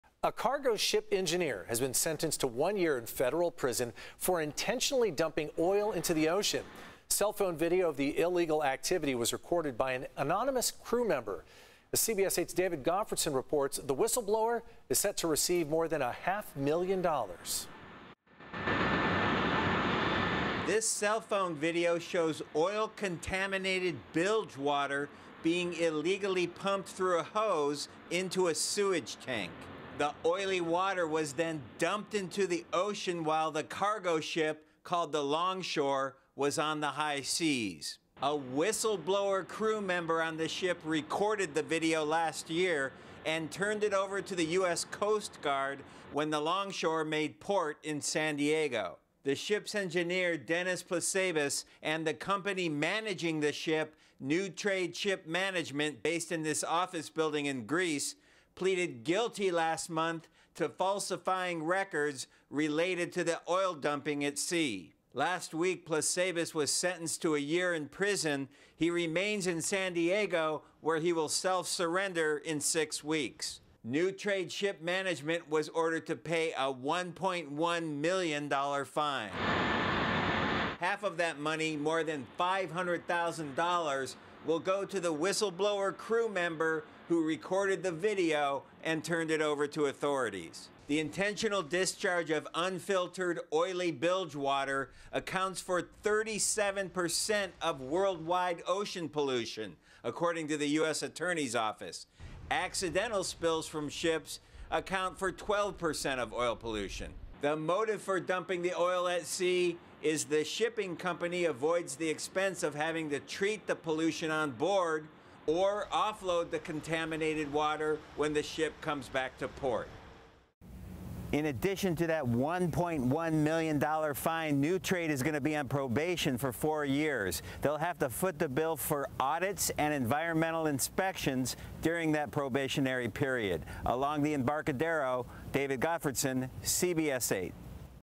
Cargo ship engineer gets one year in prison for dumping oil in ocean
A cargo ship engineer has been sentenced to one year in federal prison for intentionally dumping oil into the ocean.

Cell phone video of the illegal activity was recorded by a whistleblower crew member, who is now set to receive more than a half million dollars for reporting the incident.

The video showed oil-contaminated bilge water being illegally pumped through a hose into a sewage tank onboard the ship.  The oily water was then dumped into the ocean while the bulk cargo ship, named Longshore, was on the high seas.